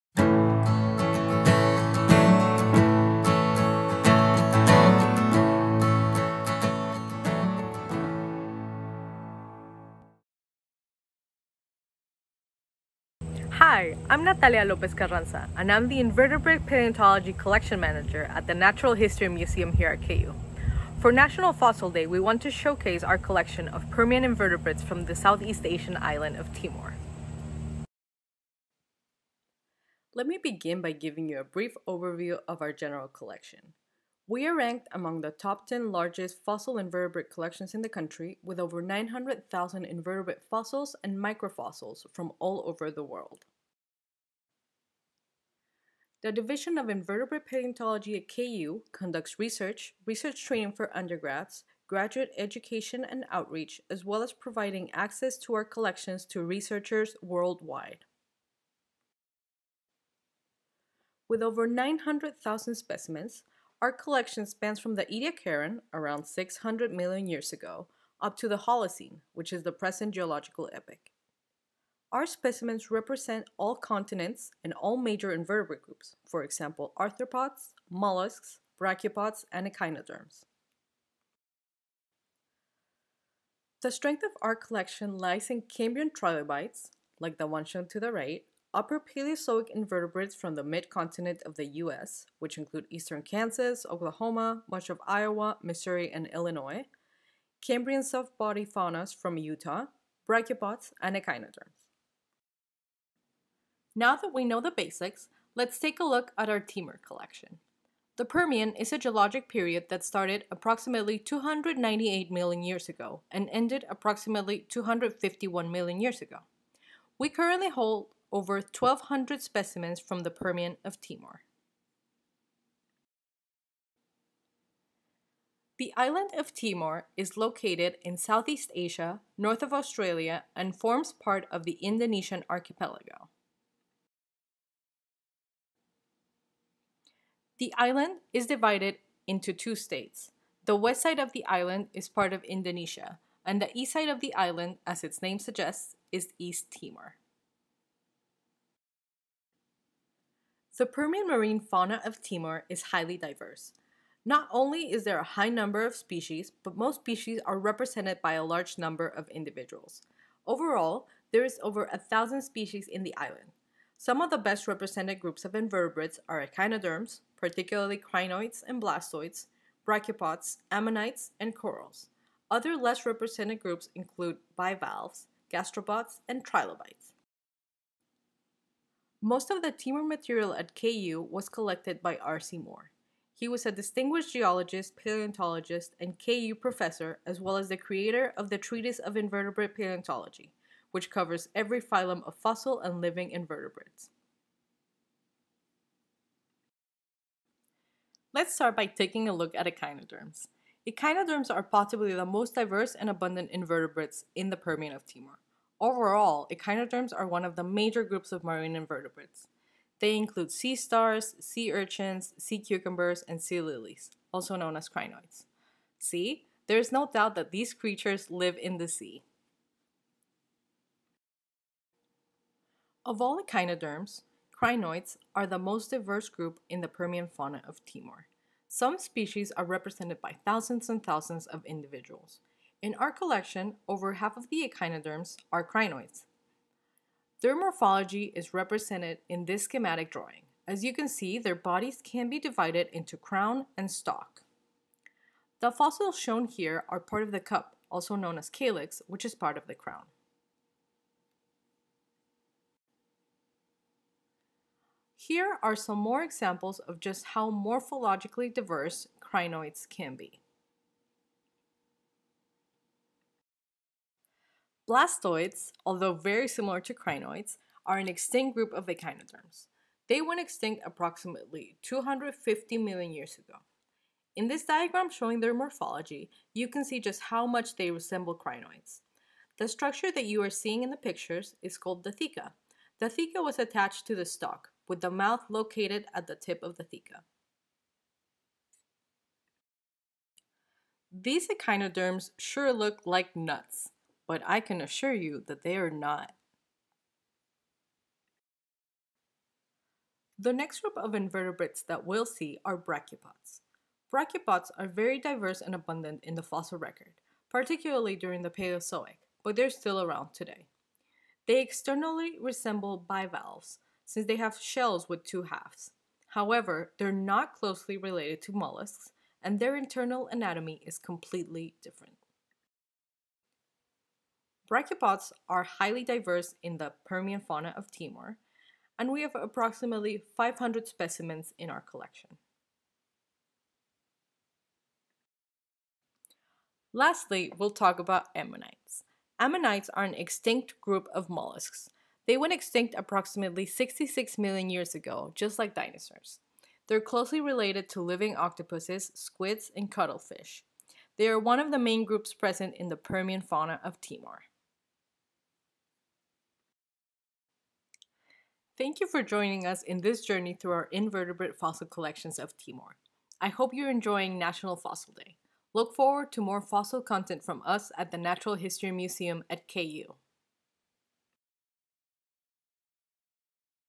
Discovery Day: National Fossil Day -- Permian Fossils of Timor at the KUNHM
Do you know the difference between a crinoid and a blastoid? You'll learn that, and lots more, in this video from the KU BI/NHM Invertebrate Paleontology Collection Manager, Dr. Natalia Lopez-Carranza.  She will wow you with intriguing invertebrate fossils that come from the Permian of the Island of Timor in Southeast Asia!